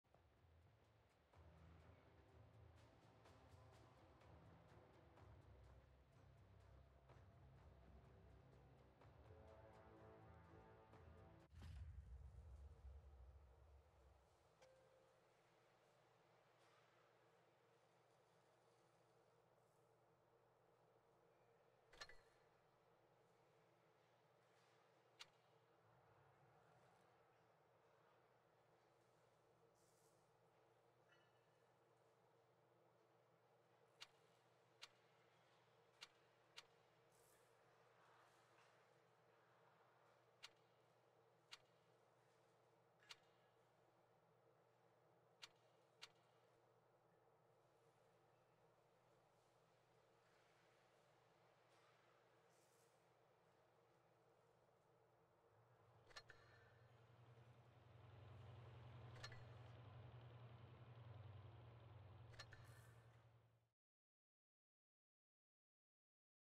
So , I can't Get The Obj.263 Legendary Camo...
Seems Like Wargaming doesn't want me to speed my money ??